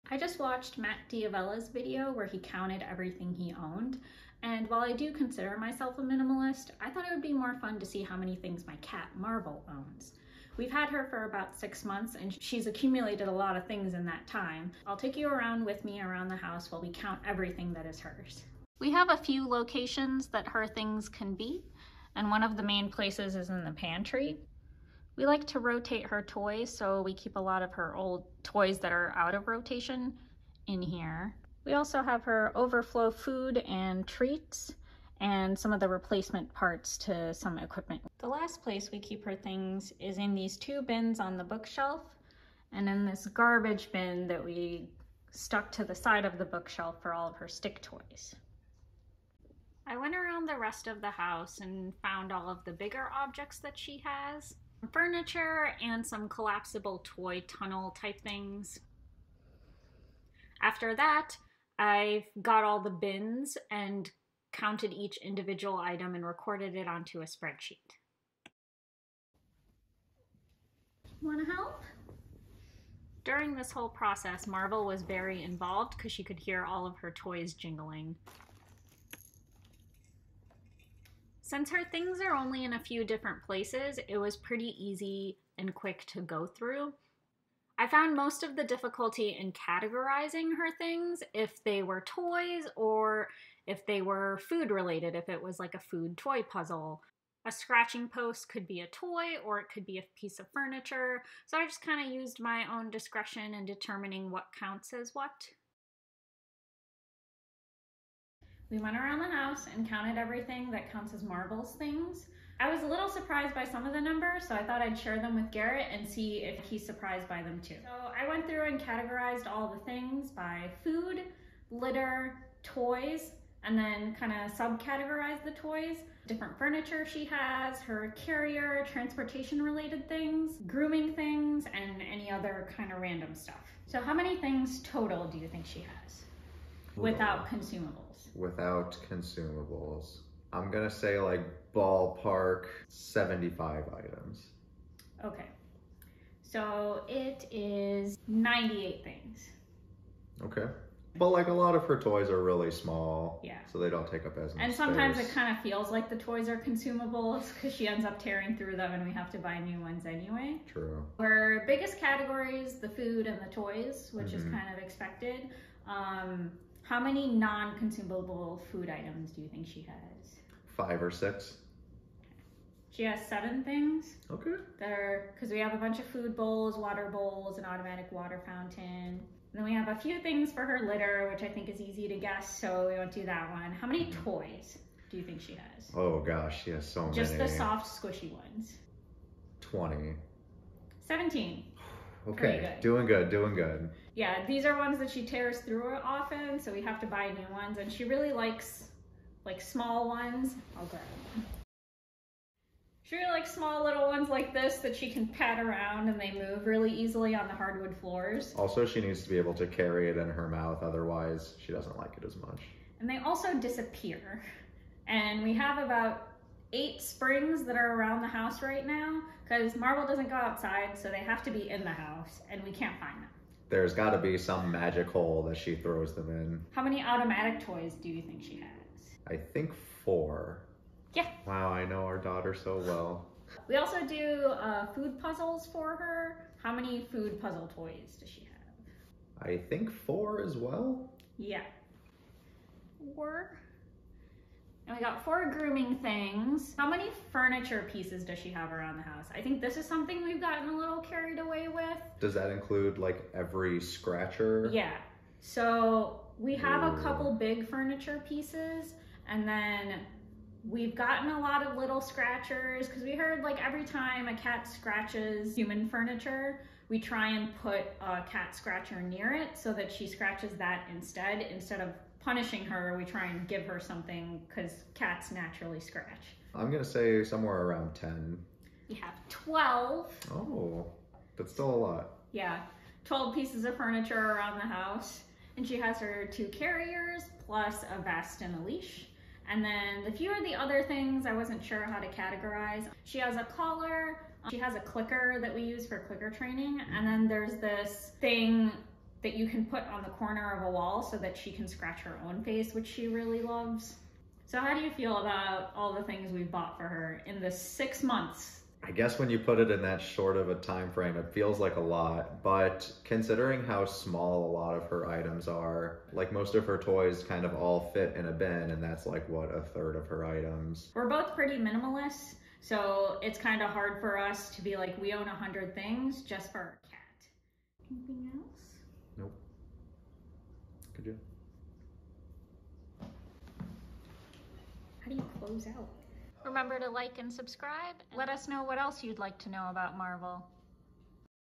I counted everything my cat owns as a minimalist.
I'm Mar-Vell, a domestic medium hair cat. I'm two years old and love to play.